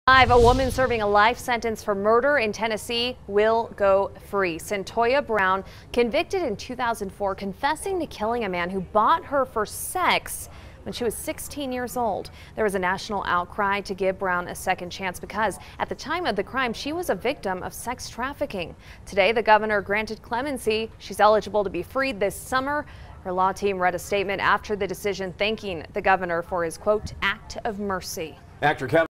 Cyntoia Brown granted clemency after 15 years in prison
A woman who says she was a 16-year-old sex trafficking victim when she killed a man in 2004 was granted clemency Monday by Tennessee’s governor and soon will be released from prison.